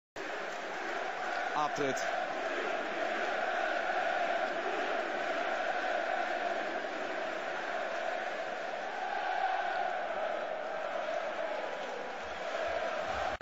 Реакция Мауриньо на красную карточку Рамиресу
Суперкубок УЕФА с Баварией. Тренер Челси Мауриньо в своем стиле